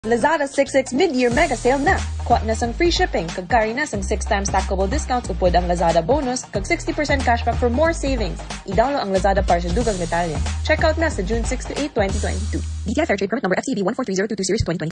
Lazada 6.6 (2022) Mid-Year Mega Sale Radio Ad Q2 2022 15s (in Hiligaynon)
This is the Hiligaynon Version of the Lazada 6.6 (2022) Mid-Year Mega Sale Radio Commercial. Check out na mula June 6 to 8, 2022!

DRPS Two 1954: Lazada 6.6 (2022) Mid-Year Mega Sale Radio Ad Q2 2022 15s (in Hiligaynon)